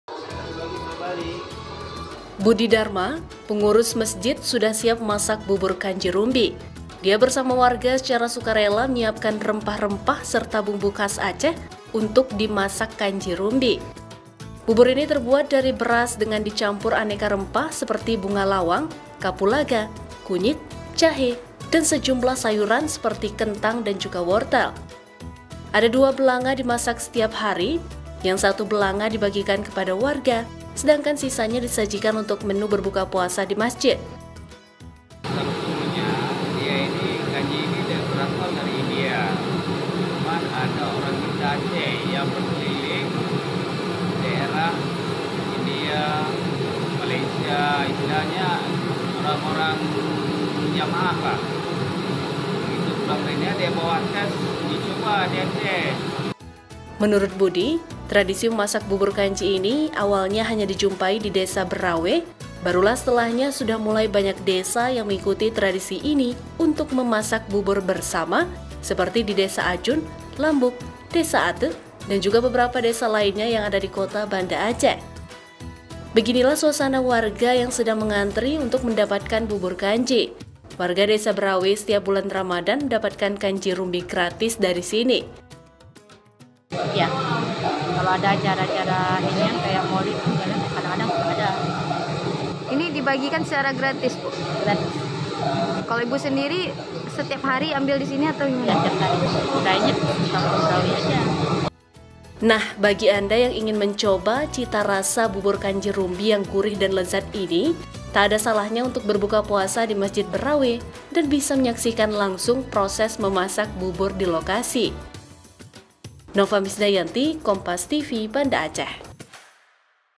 Memasak Kanji Rumbi Saat Puasa Telah Menjadi Tradisi Setiap Ramadhan
BANDA ACEH, KOMPAS.TV - Budi Darma, pengurus mesjid sudah siap memasak bubur kanji rumbi. Dia bersama warga secara sukarela menyiapkan rempah-rempah, serta bumbu khas Aceh untuk di masak kanji rumbi.

Bubur ini terbuat dari beras dengan dicampur aneka rempah seperti bunga lawang, kapulaga, kunyit, jahe, dan sejumlah sayuran seperti kentang dan wortel.

Ada dua belanga dimasak setiap hari yang satu belanga dibagikan kepada warga, sedangkan sisanya disajikan untuk menu berbuka puasa di masjid.

Menurut budi, tradisi memasak bubur kanji ini awalnya hanya dijumpai di Desa Beurawe, barulah setelahnya sudah mulai banyak desa yang mengikuti tradisi ini untuk memasak bubur bersama, seperti di desa ajun, lambhuk, desa ateuk dan beberapa desa lainnya yang ada di Kota Banda Aceh.

Beginilah suasana warga yang sedang mengantri untuk mendapatkan bubur kanji. Warga Desa Beurawe setiap bulan ramadhan mendapatkan kanji rumbi gratis.

Nah bagi anda ingin mencoba cita rasa bubur kanji rumbi yang gurih dan lezat ini tak ada salahnya untuk berbuka puasa di Mesjid Beurawe dan bisa menyaksikan langsung proses memasak bubur di lokasi.